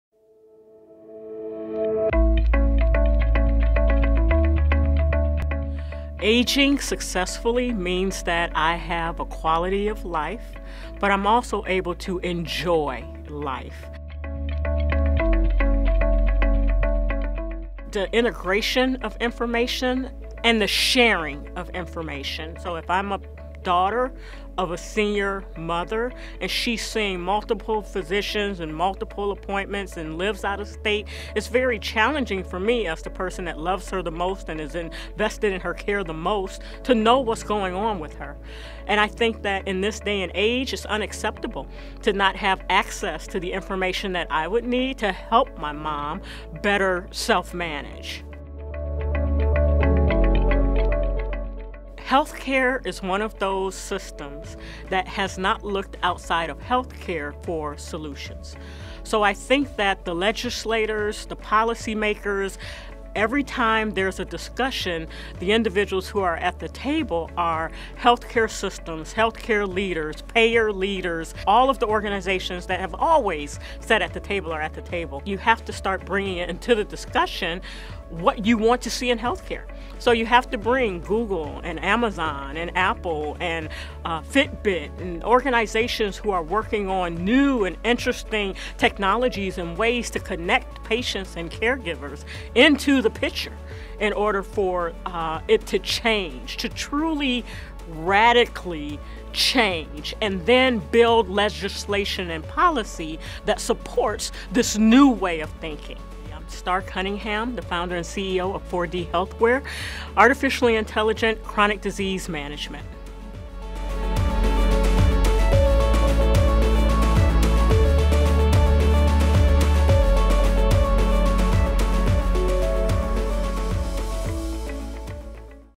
4D Healthware Founder & CEO, Star Cunningham on Successful Aging in 2030
4D Healthware Founder & CEO, Star Cunningham discusses Successful Aging in 2030 at the d.health Summit 2017.

(0:05) What will Successful Aging look like in 2030?

(0:13) What is the biggest challenge facing the Aging population and their caregivers?

(0:51) What can policymakers do now to enable Successful Aging in the future?